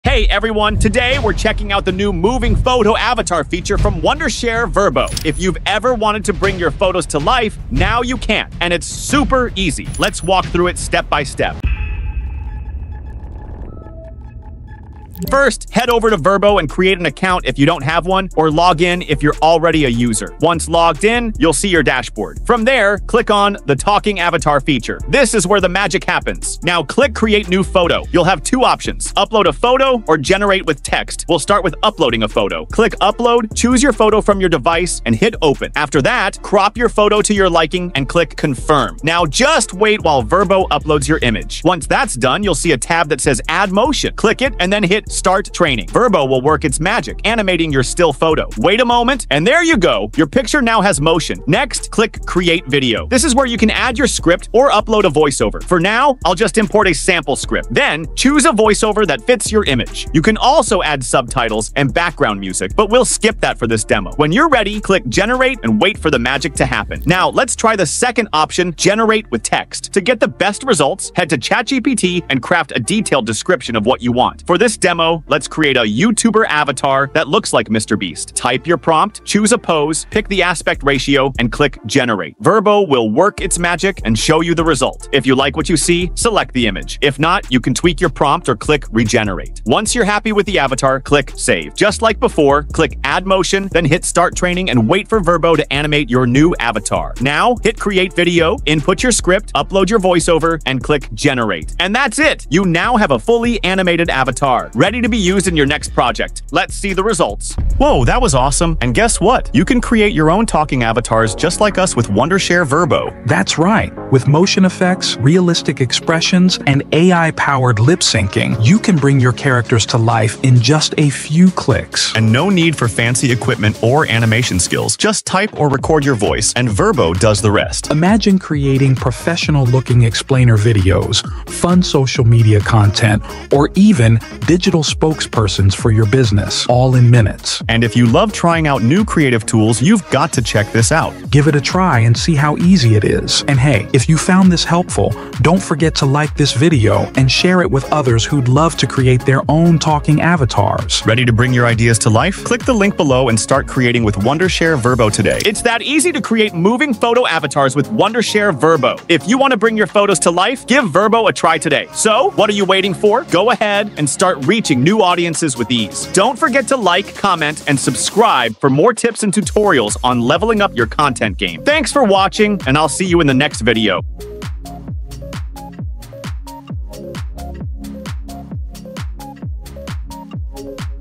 How To Create Moving Photo Avatars with Wondershare Virbo in 2 Minutes?
Say goodbye to static photos with our advanced Moving Photo Avatar technology that brings your images to life! 🌟 Our powerful ai tools enable seamless lip sync ai capabilities, creating naturally animated characters from any photo. The innovative ai animation features combined with our user-friendly photo animator app make it easier than ever to create dynamic, engaging content. 💫

If you found this video helpful 🙌！Please remember to like this video👍or leave a comment 💬 below!

⏰Timeline:
0:00 Intro
0:15 Create a Virbo account
0:23 Head to Talking Photo
0:33 Method 1: Upload a photo and Add motion
1:08 Method 2: Generate photo with text and Add motion
1:47 How's Moving Photo Avatar like?
2:35 Create your moving photo avatar with Wondershare Virbo, bringing your photo to life.